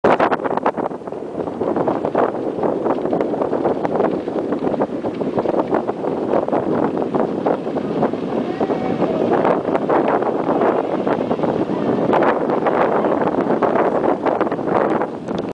Horses Running
Filming of Kia commercial in Utah.
Sequence of horses running on a wet ground
next to the Colorado River.